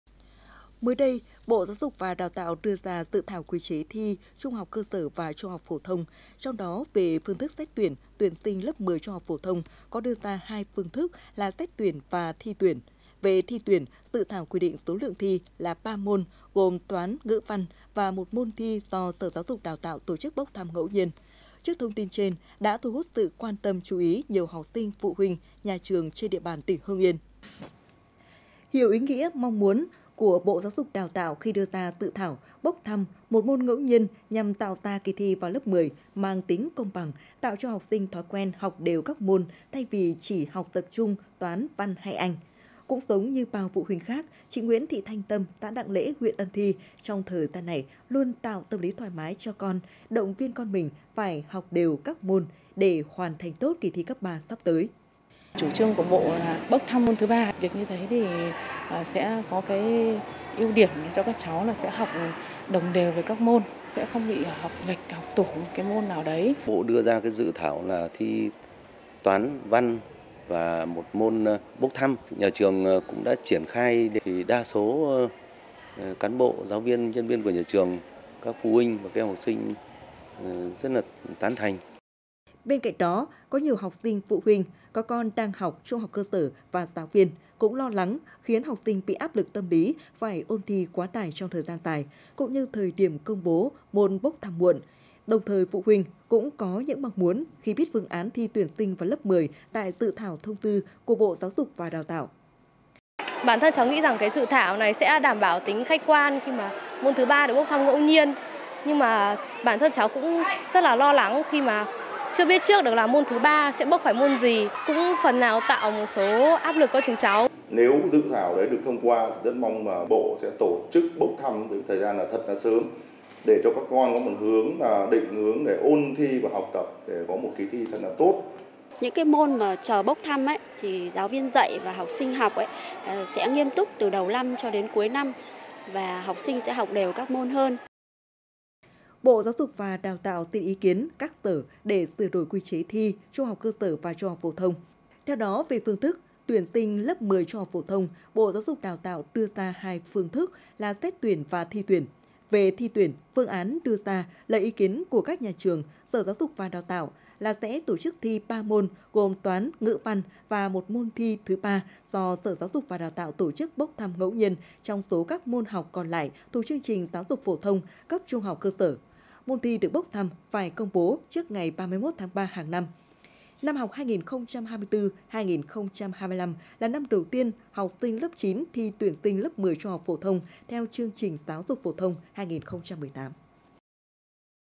Bộ GD và ĐT đề xuất bốc thăm môn thi thứ 3 vào lớp 10, phụ huynh, học sinh và giáo viên nói gì?
Hưng Yên TV | Bộ GD và ĐT đề xuất bốc thăm môn thi thứ 3 vào lớp 10, phụ huynh, học sinh và giáo viên nói gì?

Tiktok: Truyền hình Hưng Yên
Facebook: Đài Phát thanh và Truyền hình Hưng Yên
Zalo: Đài PTTH Hưng Yên